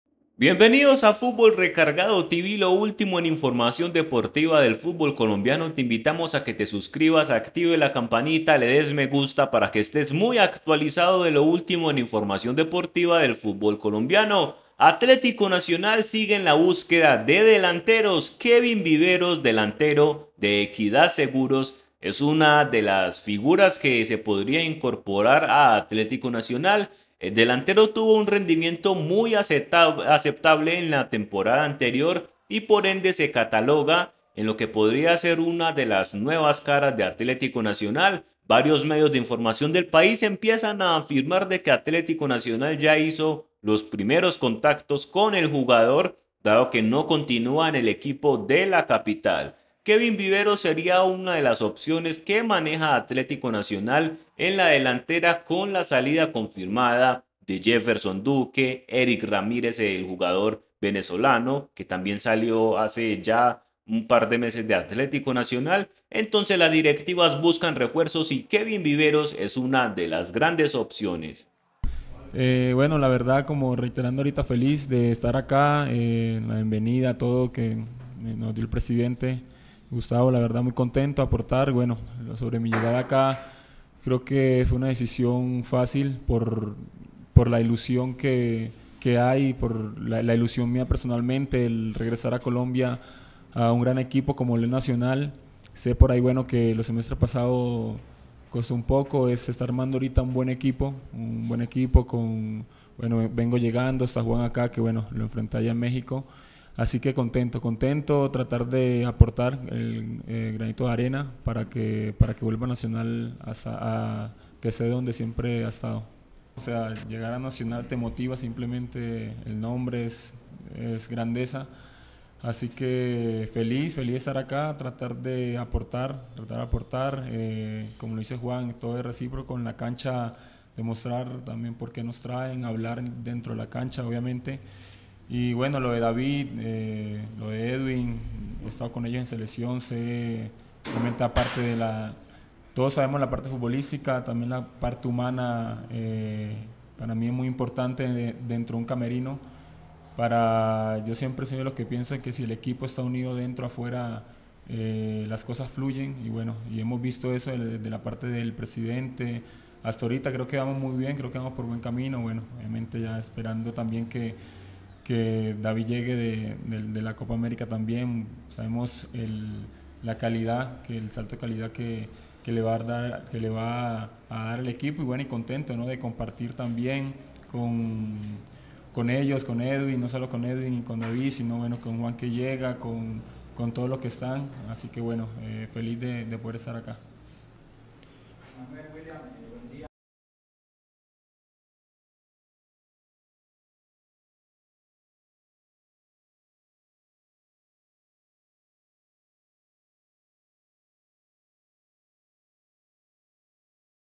🔥😨NUEVO DELANTERO ATLETICO NACIONAL ¿KEVIN VIVEROS? ES LA GRAN OPCION QUE MANEJAN.
😨🔥 KEVIN VIVEROS OPCION EN ATLETICO NACIONAL